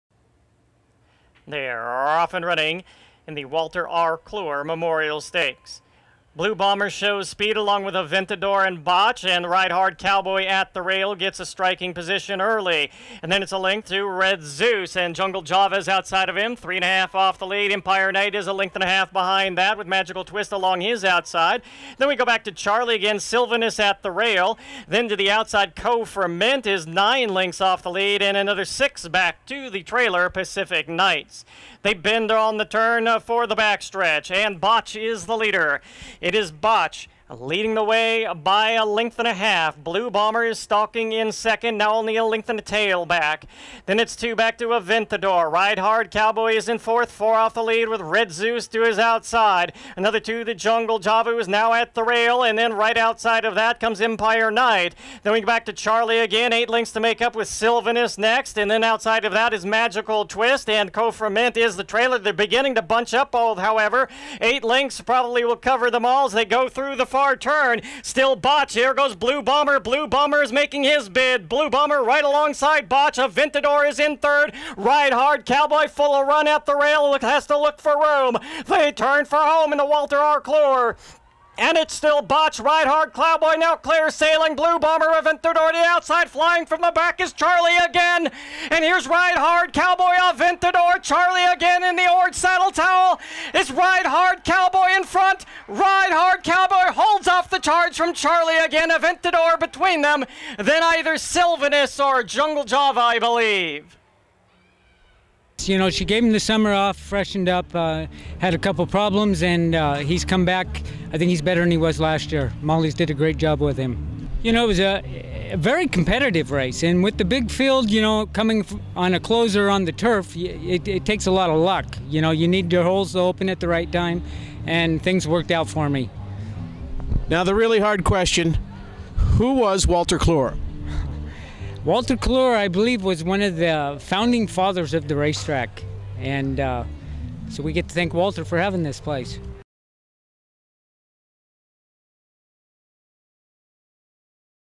Walter Cluer Memorial Stakes
RIDE HARD KOWBOY wins the Walter R. Cluer Memorial Stakes.
Purse $30,000. One Mile-Turf  1:35.0. Turner Shiew & Robert Morgan, Owners. Molly Pearson, Trainer. Scott Stevens, Jockey